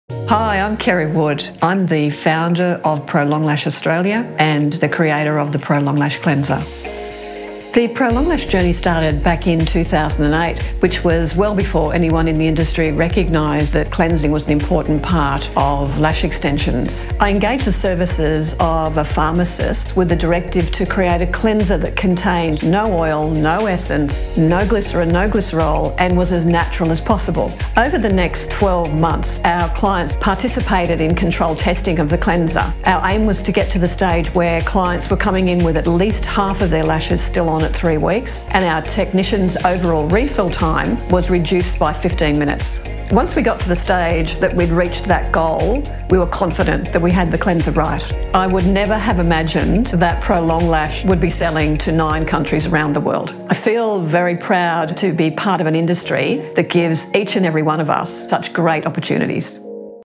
Prolong Lash : Our 10-YEAR JOURNEY in just 60 seconds
From humble beginnings to becoming a leader in the eyelash extension industry, Prolong Lash worked tirelessly to create high-quality lash products that make women feel confident and beautiful.

Discover ProlongLash on socials and stay on top of our news, tips and much more!